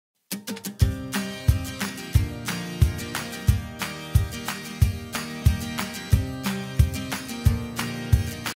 Crazy...NEW, simple umbrella anchor installation
A child with a broken finger can install this beach umbrella anchor in less than 8 seconds...nothing else like it